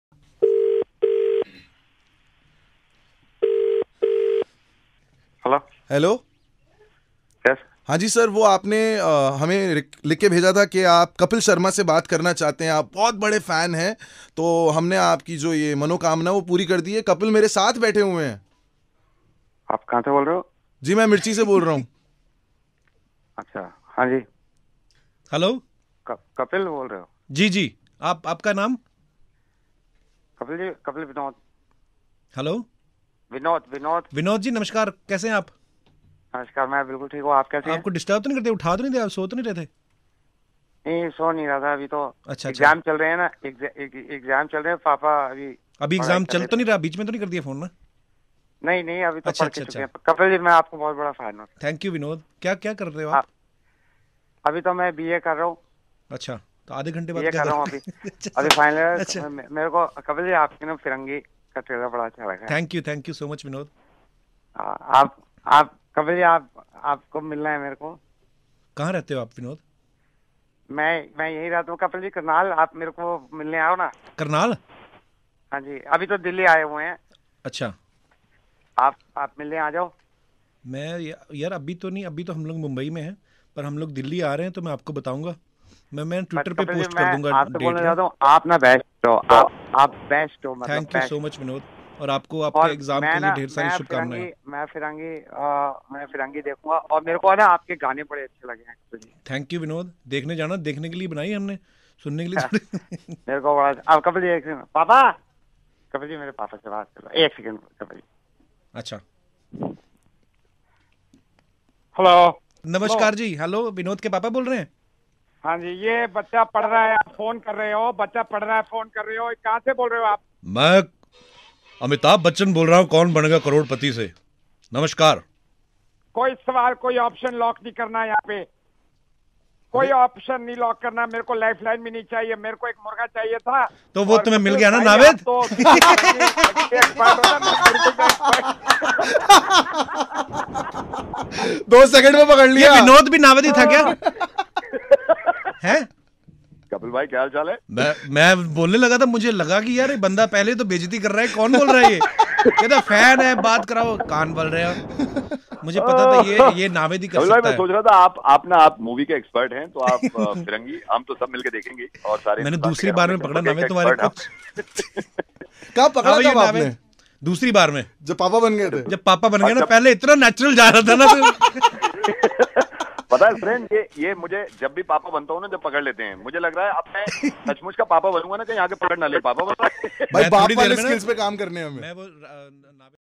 Rj Naved Prank with Kapil Sharma
Rj Naved made a prank call to Kapil Sharma in his studio.